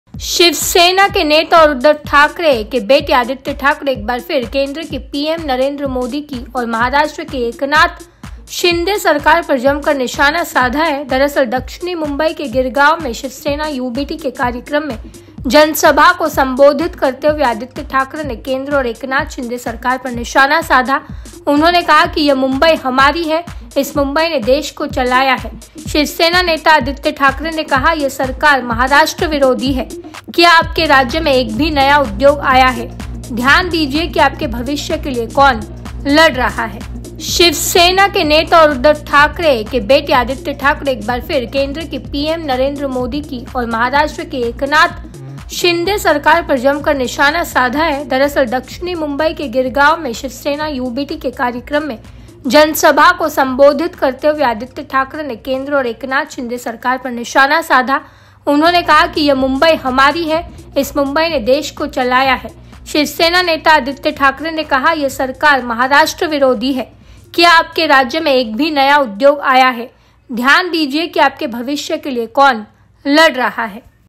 Aditya Thackeray ने केंद्र और एकनाथ शिंदे सरकार पर निशाना साधा
शिव सेना (यूबीटी) (Shiv Sena UBT) के नेता और उद्धव ठाकरे (Uddhav Thackeray) के बेटे आदित्य ठाकरे (Aaditya Thackeray) एक बार फिर केंद्र की  पीएम नरेंद्र मोदी (PM Narendra Modi) की और महाराष्ट्र (Maharashtra) की एकनाथ शिंदे (Eknath Shinde) सरकार पर जमकर निशाना साधा है.